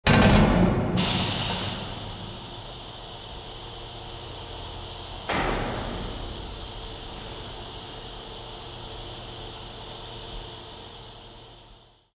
Large Hydraulic Cargo Door Unlocks and Opens
Large Hydraulic Cargo Door Unlocks and Opens · Sound Ideas

Doors: Extreme